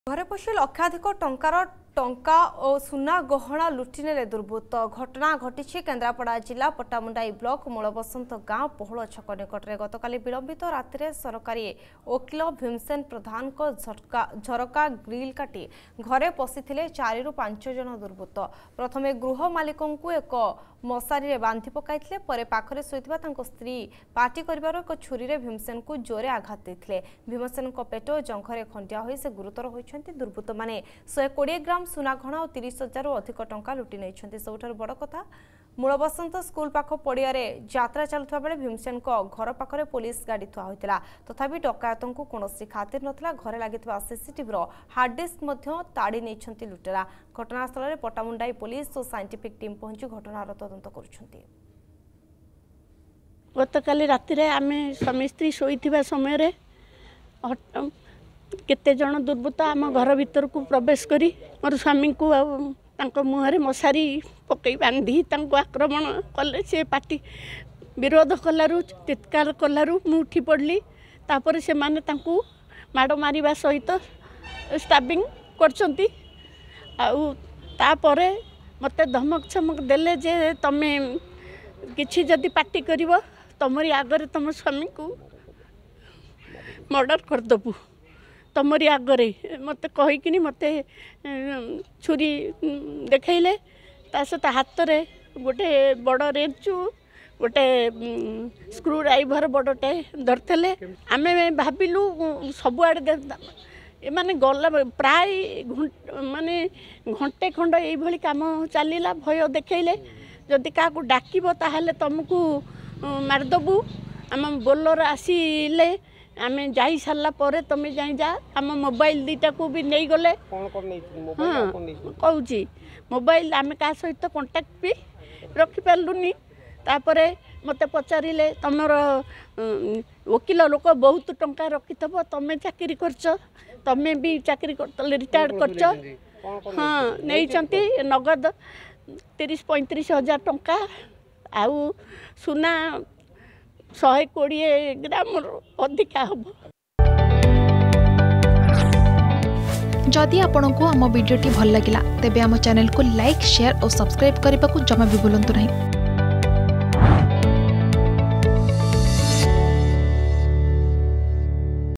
ଘରେ ପଶି ଲକ୍ଷାଧିକ ଟଙ୍କାର ଟଙ୍କା ଓ ସୁନା ଗହଣା ଲୁଟିନେଲେ ଦୁର୍ବୃତ୍ତ || Theft || Kendrapada
ଘରେ ପଶି ଲକ୍ଷାଧିକ ଟଙ୍କାର ଟଙ୍କା ଓ ସୁନା ଗହଣା ଲୁଟିନେଲେ ଦୁର୍ବୃତ୍ତ । ଘଟଣା ଘଟିଛି କେନ୍ଦ୍ରାପଡ଼ା ଜିଲ୍ଲା ପଟ୍ଟାମୁଣ୍ଡାଇ ବ୍ଲକ ମୂଳବସନ୍ତ ଗାଁ ପୋହଳ ଛକ ନିକଟରେ ।